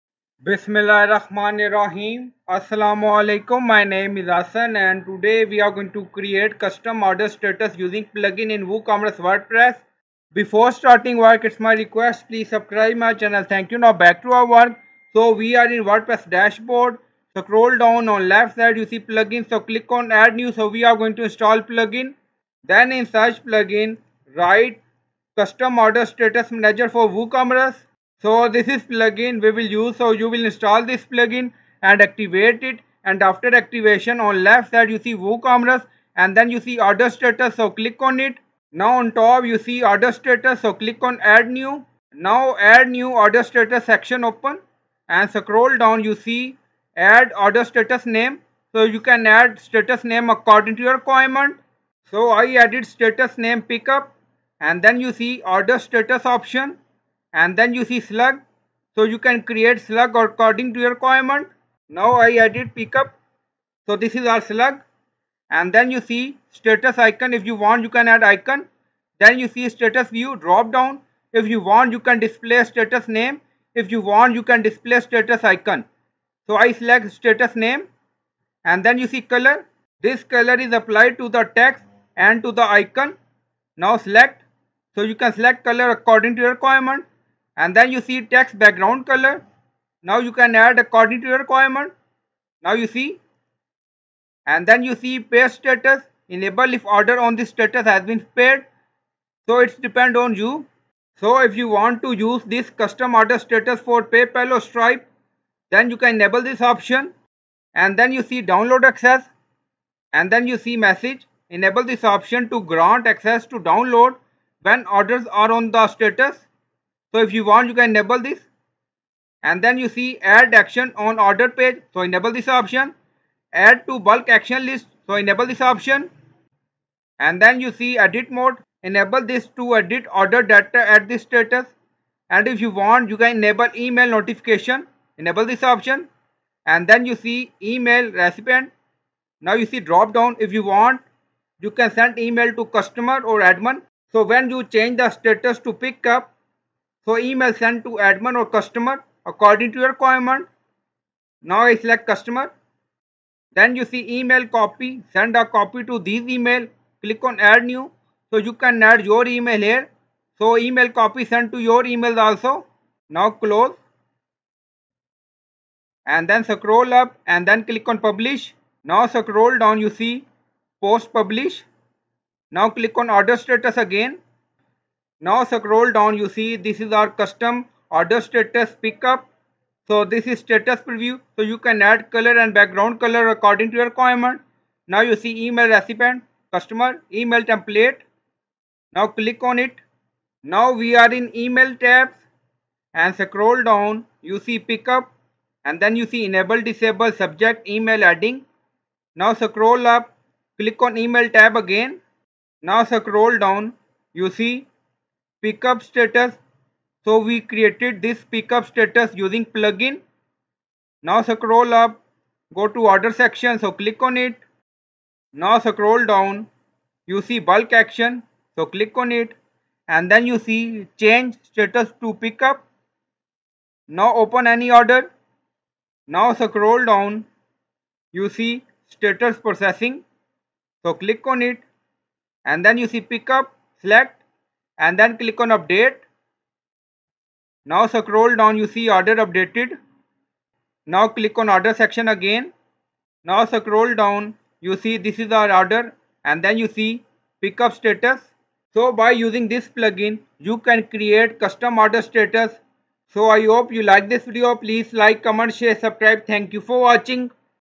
How to Create Custom Order Status Using Plugin in WooCommerce WordPress
In this woocommerce tutorial for beginners you will learn how to create custom order statuses using plugin " Custom Order Status Manager for WooCommerce " in wordpress website. So you can create orders status according to your requirement.